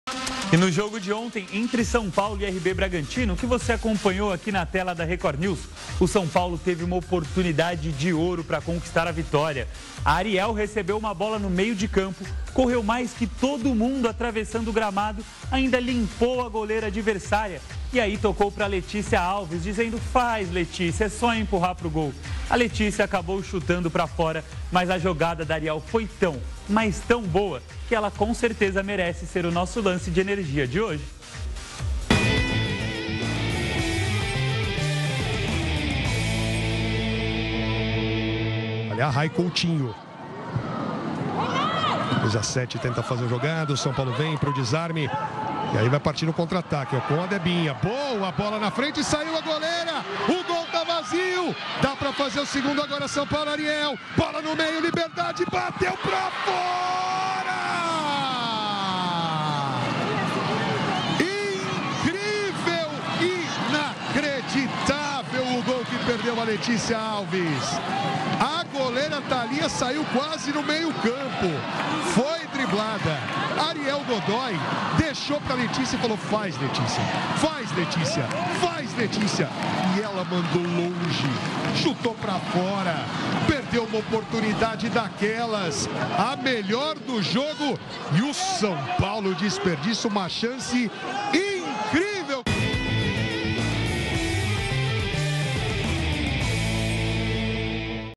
Lance de Energia: jogada de Ariel é destaque em Red Bull Bragantina x São Paulo
O Lance de Energia da partida entre Red Bull Bragantina x São Paulo pelo Campeonato Paulista Feminino, exibido ontem (04) na Record News é a jogada da Ariel que recebeu a bola no meio de campo e driblou até a goleira, tocando para a Letícia Alves que desperdiçou a chance e não empatou para o tricolor.